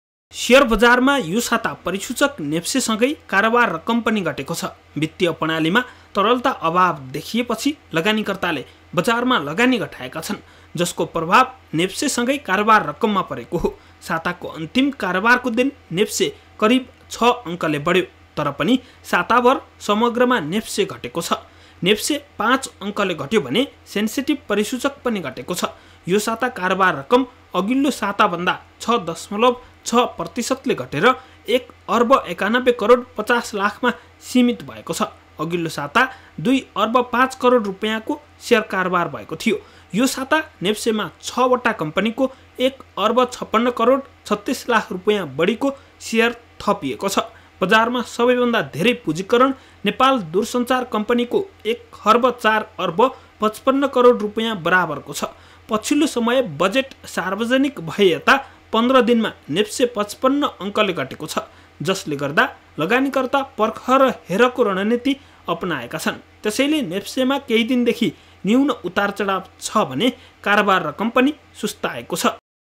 शेयर बजारमा कारोबार घट्यो, लगानीकर्ताले खरिदचाप घटाए - NEWS24 TV
लगानीकर्ताले खरिद चाप घटाएपछि यो साता शेयर बजारमा गिरावट आएको छ । बजारमा खरिदकर्ता भन्दा विक्रीकर्ता हावि देखिए । जसका कारण नेप्से घट्यो भने कारोबार रकममा पनि गिरावट आएको छ ।